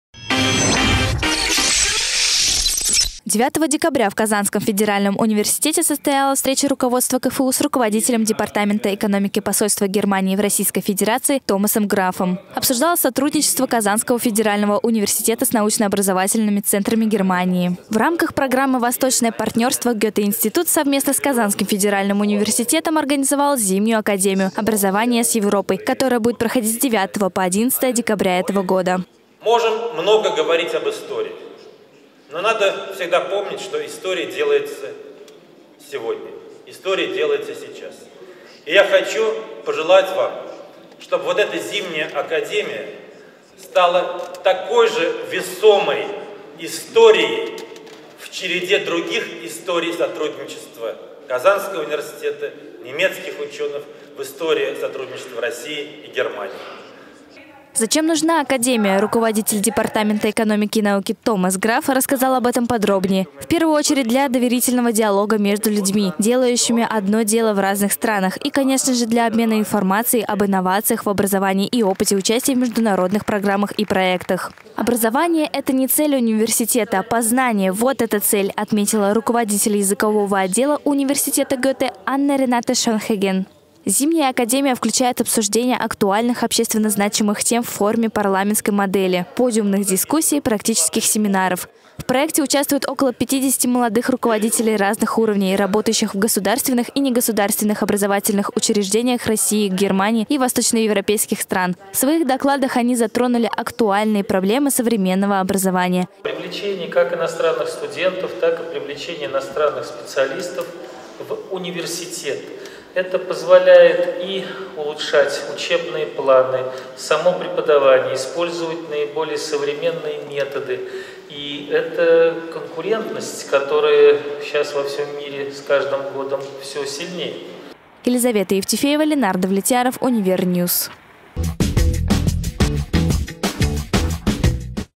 ОБРАЗОВАНИЕ С ЕВРОПОЙ
Европейский союз стремится расширить сотрудничество со странами Восточной Европы, в том числе и в сфере образования. Так в Казанском федеральном университете состоялось открытие Зимней Академии «Образование с Европой».